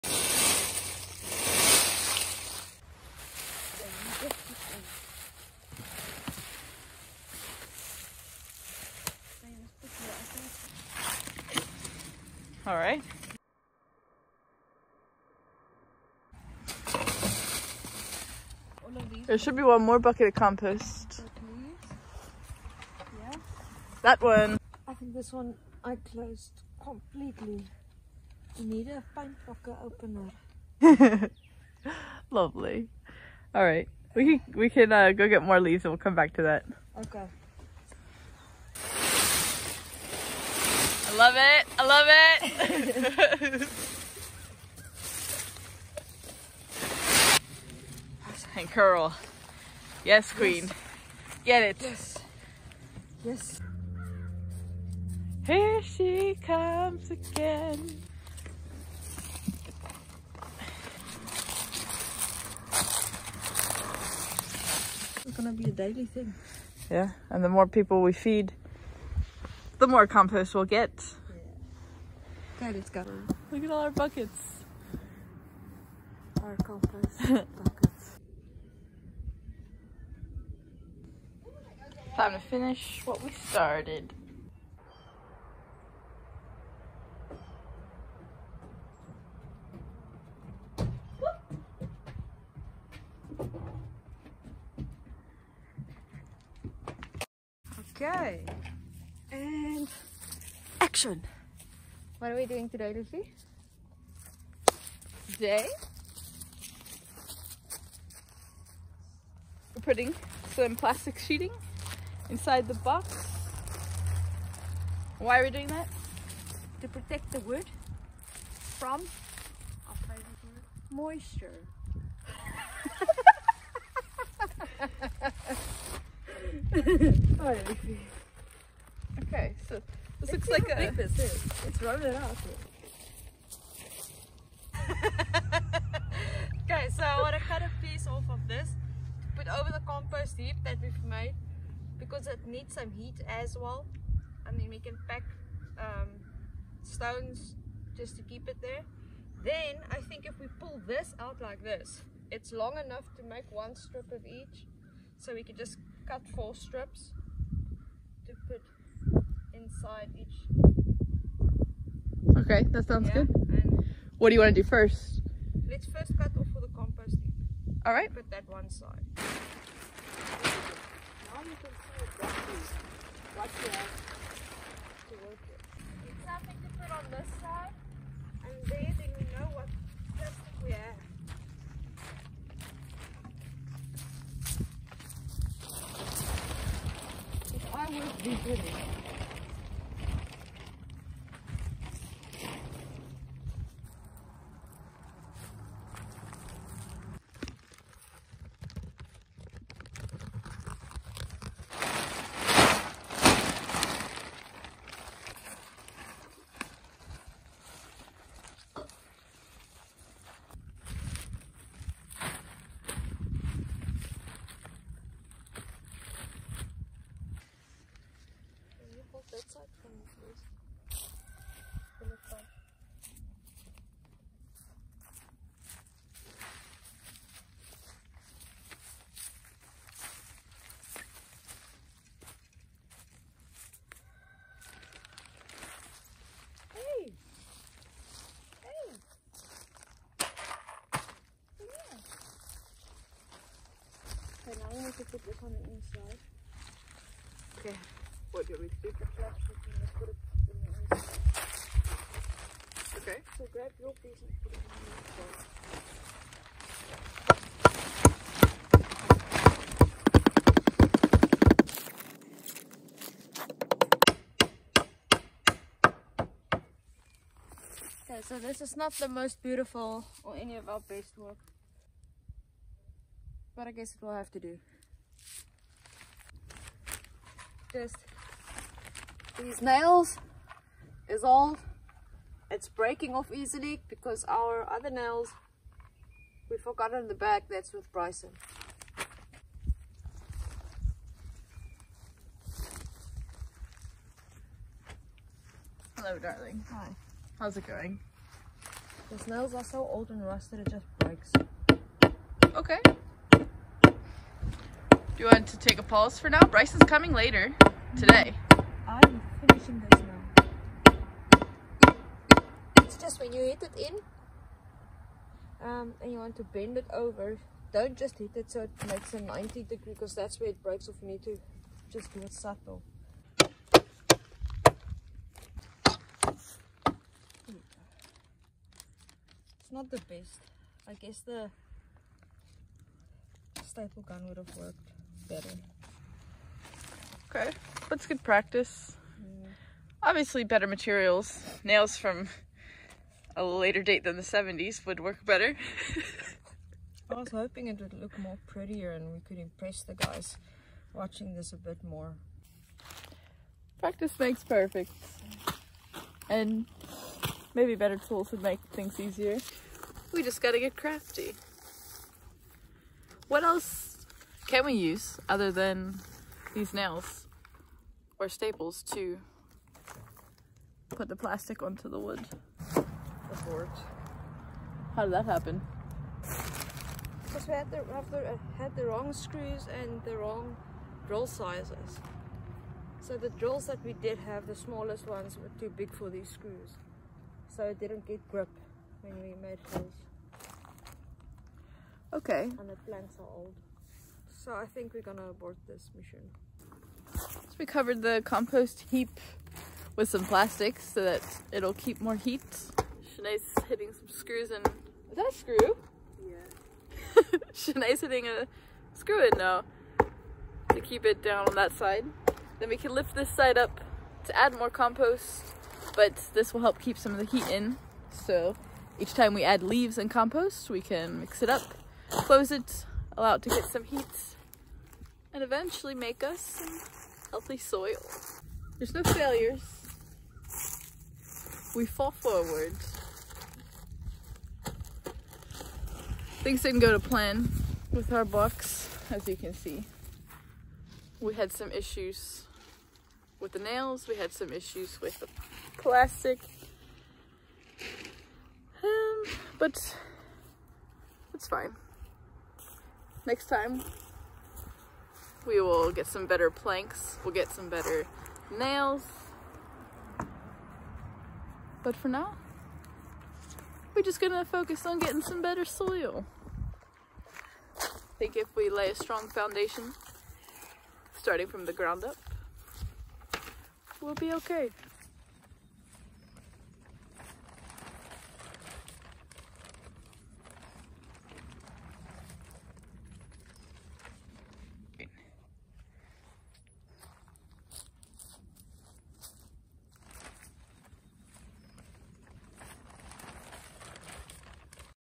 Homemade garden box failure // composting success!
Our goal is to grow food for ourselves and communities throughout the world one day.

Today we start with one garden in Pennsylvania and a dream to build The Enough Garden in South Africa when we return! 🇺🇸🇿🇦

We tried to build a garden box with wood pallets that Carenna’s brother had behind their parents house.

Those pallets may have ranked poor in quality but high in value as we practiced our first building project together.

We didn’t get the result we hoped for, but know we will be learning a lot very soon! A local teacher is willing to teach and guide us through our next project…

THE SHED!

Does anyone have tips for building garden boxes or other objects to plant in?

We are thinking about tires!